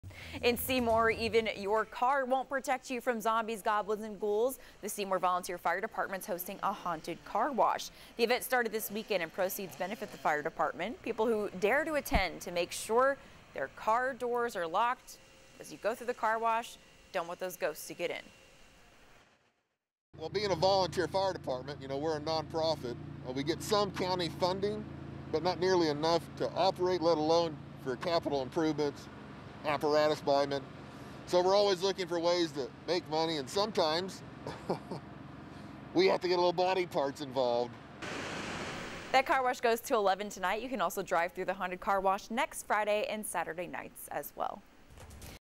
Locally owned Seymour business hosts "Haunted Car Wash" Saturday night
Tickets cost $25 per vehicle for people to go through Auto Spa Express' haunted car wash.